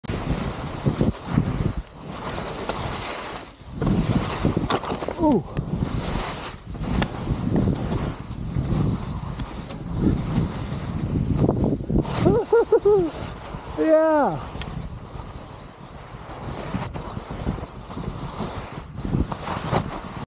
The Reverse Wiggle at Alta, UT on July 19, 2023
Daryn Edmunds and Miles Clark on The Reverse Wiggle at Alta, UT on July 19, 2023